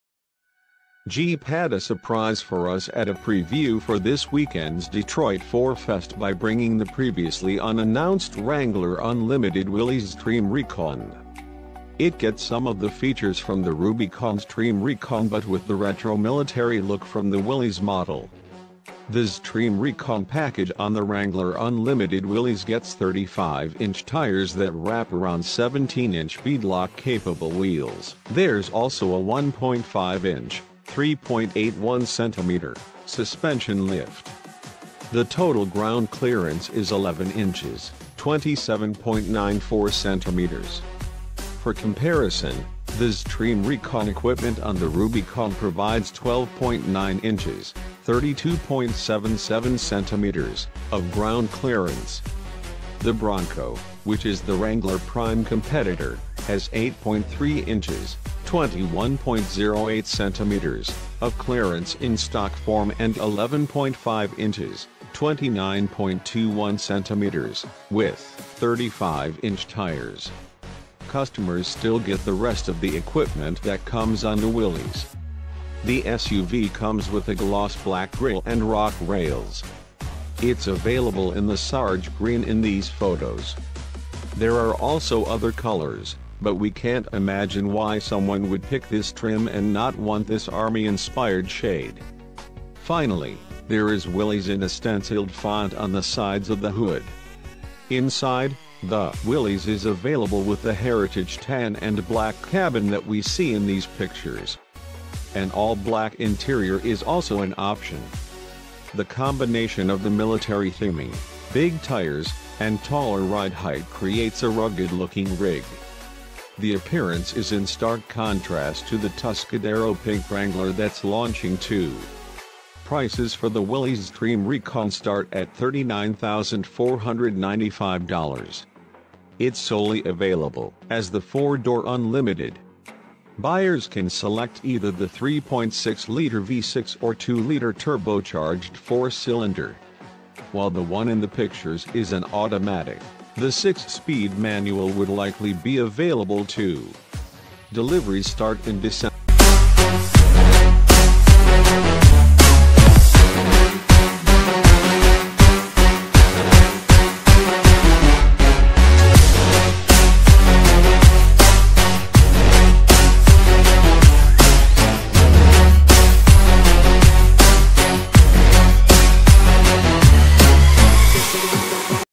Jeep Unveils Wrangler Unlimited Willys Xtreme Recon With Army Looks
Jeep had a surprise for us at a preview for this weekend's Detroit 4Fest by bringing the previously unannounced Wrangler Unlimited Willys Xtreme Recon. It gets some of the features from the Rubicon Xtreme Recon but with the retro military look from the Willys model.